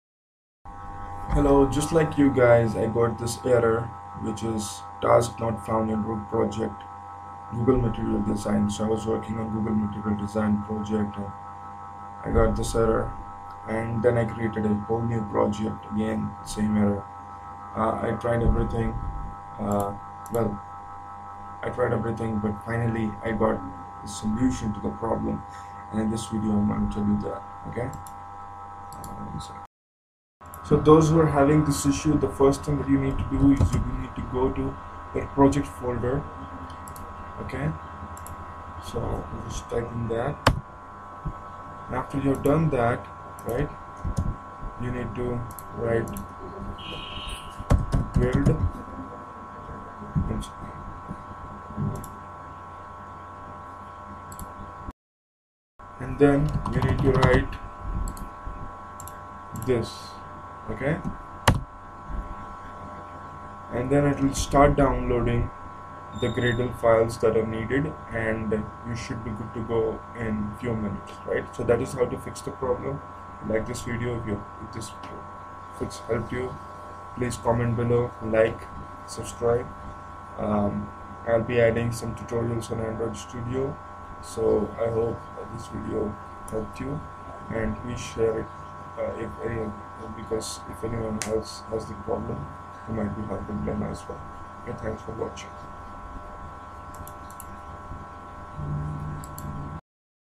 Task '' not found in root project android studio FIX
Error:FAILURE: Build failed with an exception.

* What went wrong:
Task 'XXX' not found in root project 'GoogleMaterialDesign'.

* Try:
Run gradle tasks to get a list of available tasks. Run with --stacktrace option to get the stack trace. Run with --info or --debug option to get more log output.